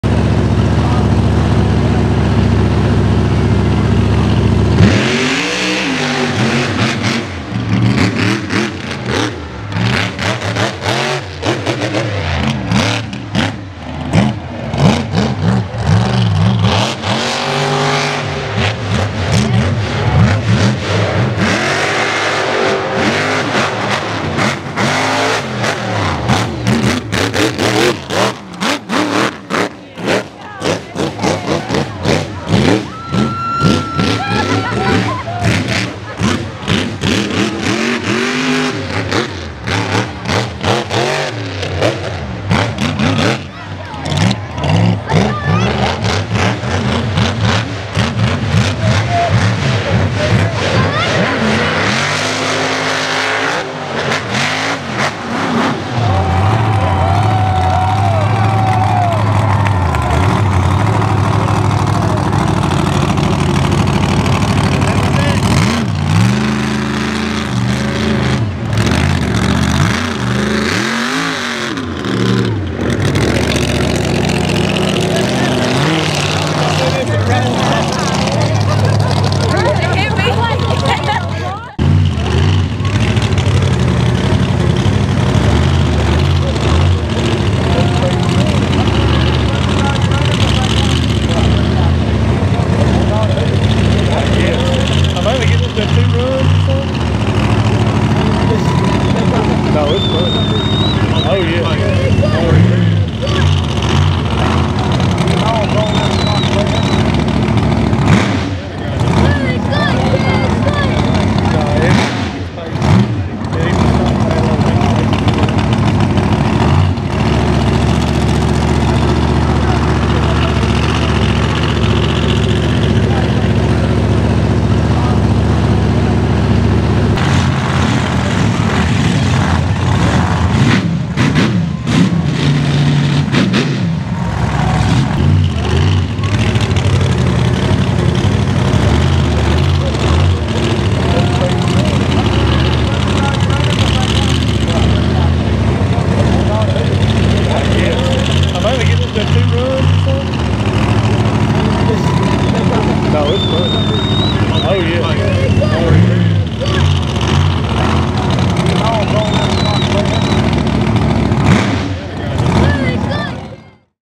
Dennis Anderson's KING SLING MEGA TRUCK!
Check out the insane King Sling Mega Mud Truck driven by Dennis Anderson. See it in action and then see a walk around of this beast with the skins on and off at Dallas Ga Mud Bog.